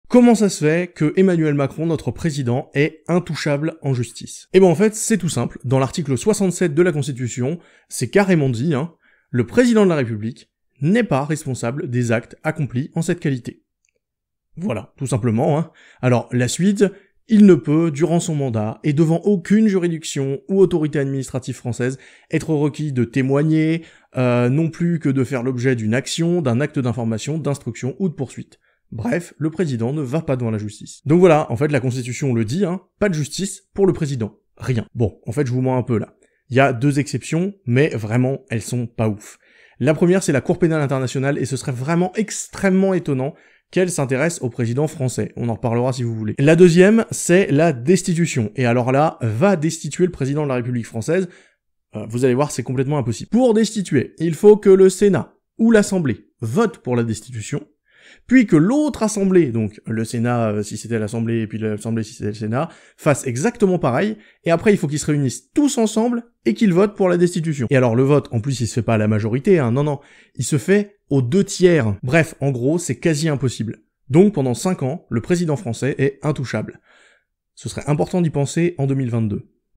Pourquoi Macron est intouchable ?
On va parler du statut du Président par rapport à la justice. En un mot : l’impunité.